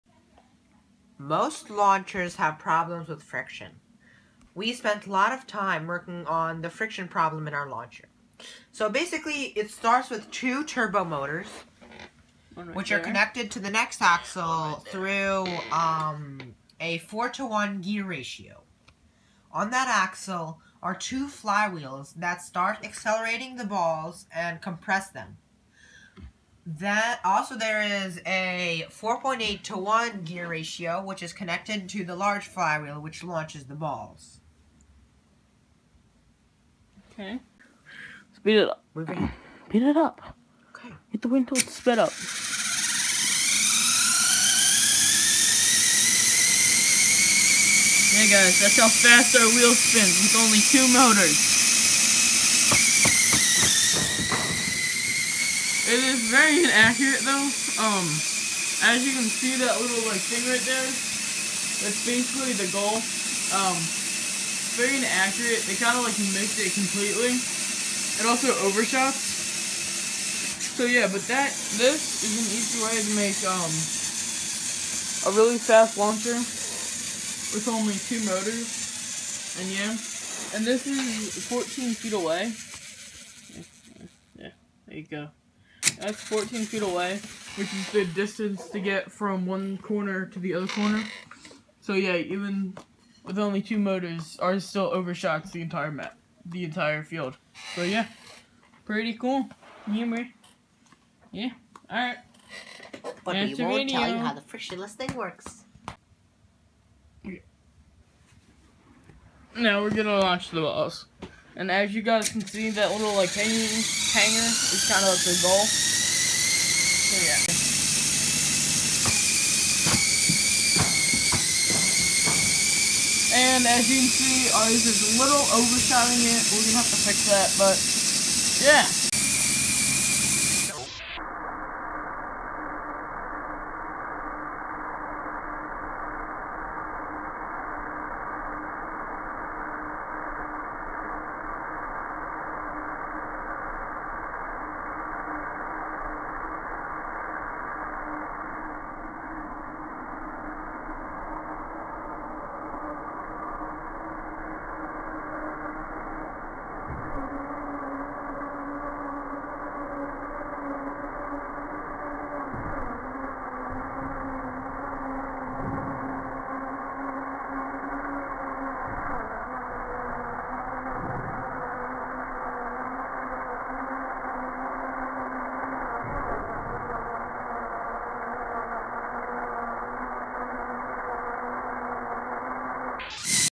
VEX NbN low-friction launcher prototype
VEX Nothing but Net - single flywheel low-friction launcher prototype. It can launch 4 balls in two seconds using two motors at full power.